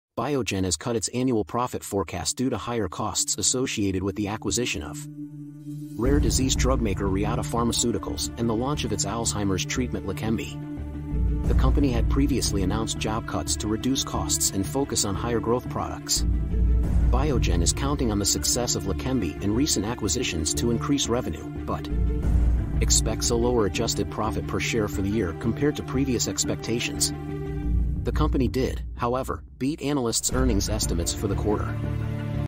Biogen cuts annual profit forecast on persistent higher costs...
Biogen has cut its annual profit forecast due to higher costs associated with the acquisition of rare disease drugmaker Reata Pharmaceuticals and the launch of its Alzheimer's treatment Leqembi. The company had previously announced job cuts to reduce costs and focus on higher-growth products. Biogen is counting on the success of Leqembi and recent acquisitions to increase revenue, but expects a lower adjusted profit per share for the year compared to previous expectations. The company did, however, beat analysts' earnings estimates for the quarter.

View the original article (Reuters)
Full title: Biogen cuts annual profit forecast on persistent higher costs

Questions or Suggestions? Check our email on our 'about' page.